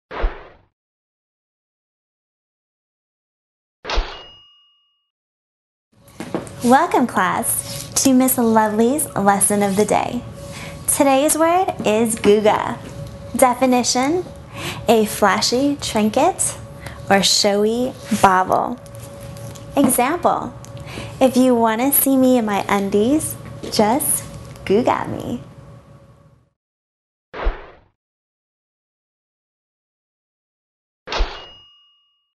Jayd Lovely Word of the Day - Googa
Jayd Lovely gives you your word of the day Sylk style... Todays word is Googa.